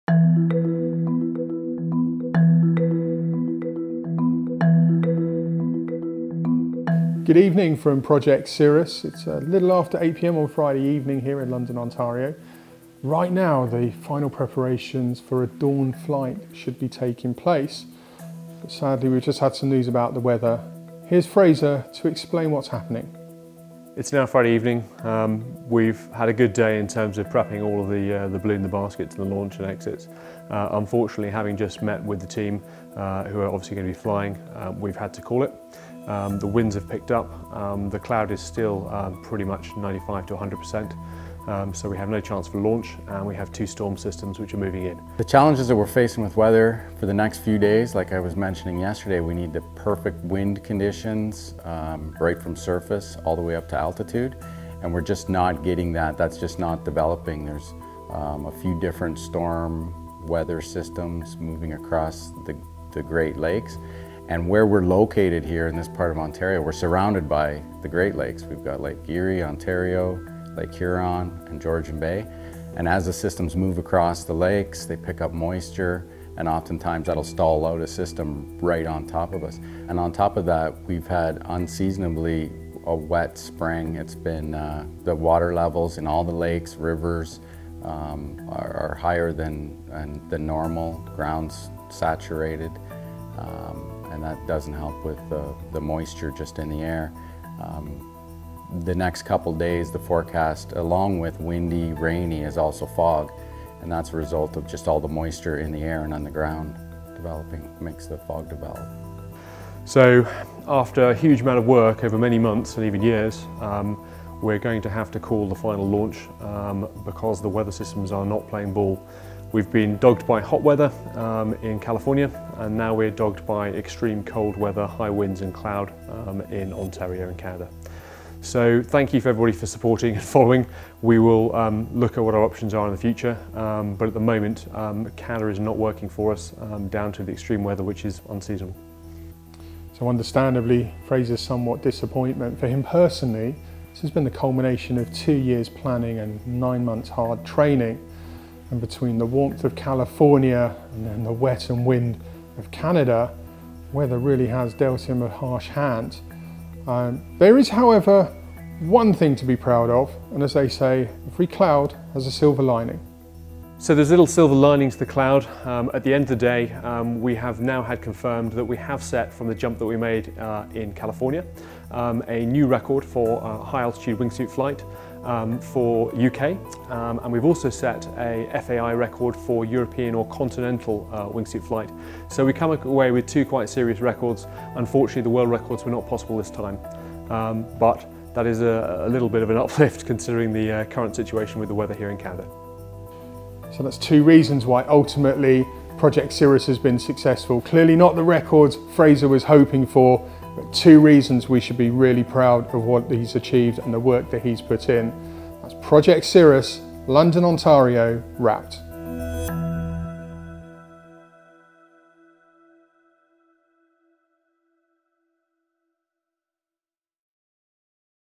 PROJECT CIRRUS: Diary, Canada Wrap Up
Jump day - but atrocious weather prevents the team getting off the ground.